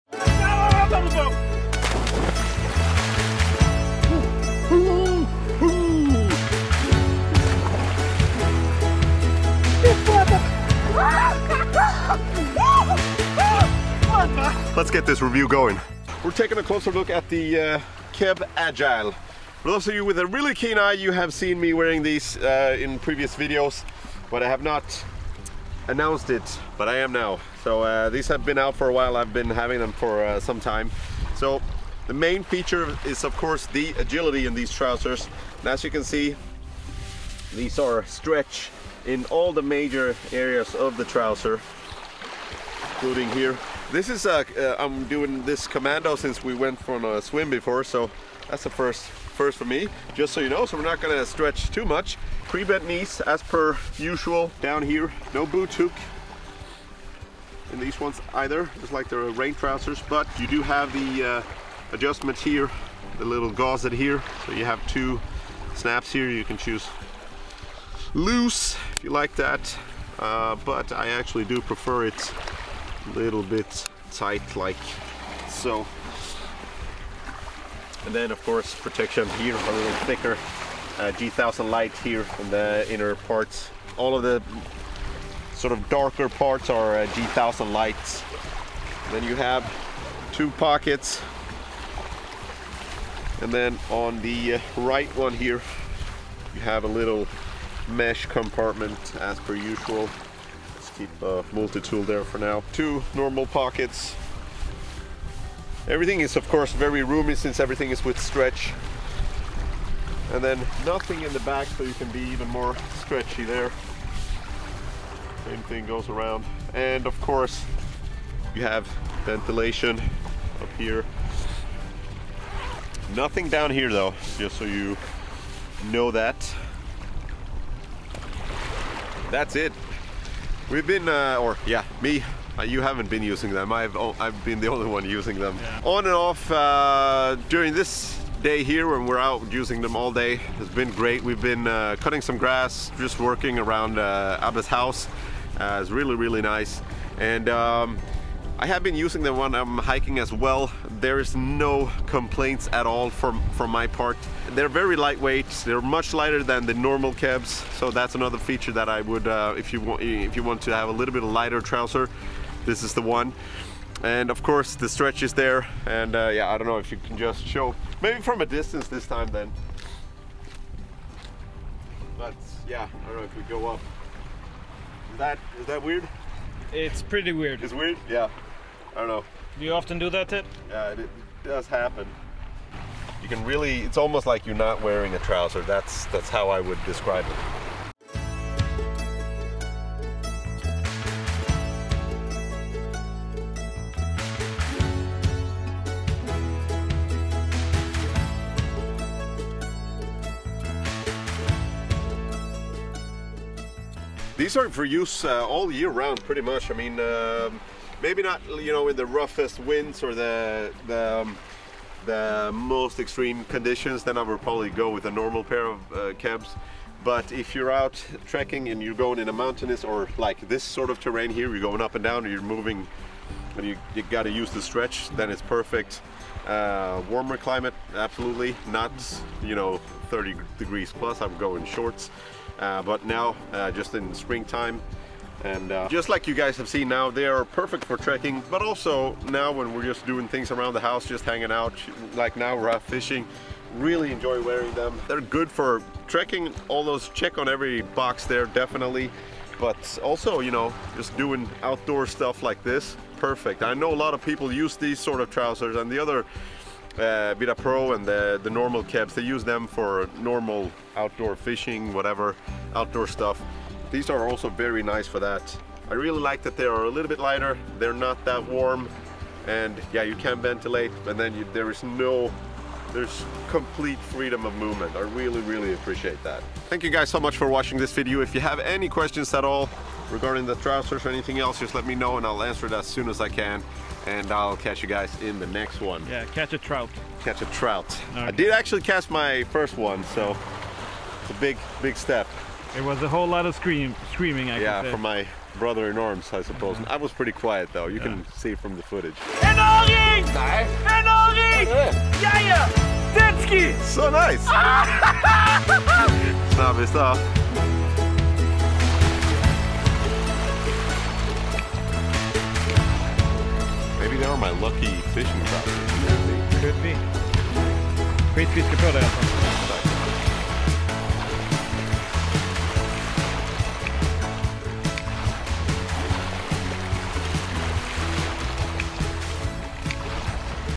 NEW! Fjällräven Keb Agile - Comprehensive test and review by a leg model
If you can't get enough of manly screams watch til the end! :)

The keb Agile is precisely what the say they are, I've been giving these trousers a run for the money now during the winter and spring, I enjoy the freedom of movement and that they are light yet durable, let me know what you guys think!

PRODUCT DESCRIPTION
Created to be your favourite all-round trekking trousers from spring to autumn, Keb Agile Trousers combine low weight and high movability in a hard-to-resist format. Made from a durable, double-woven stretch fabric that dries quickly and protects legs from rough terrain. The design is minimalistic with tapered legs, and the leg pockets and knees are in G-1000 Lite Eco Stretch for extra durability. Perfect for treks where you need excellent freedom of movement combined with durability and protection.

The drone I use
Main camera
24 MM lens